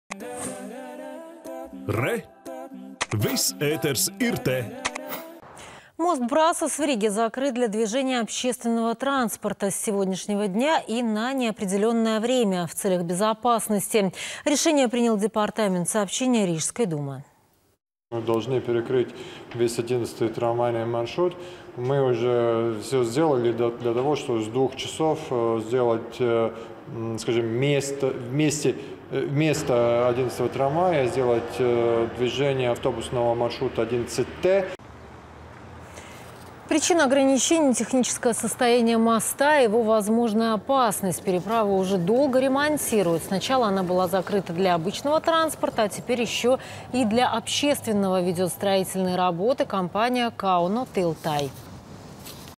Мост брасас закрыт для транспорта
Мост Брасас в Риге закрыт для движения общественного транспорта. С сегодняшнего дня и на неопределенное время. В целях безопасности. Решение принял департамент сообщения Рижской думы.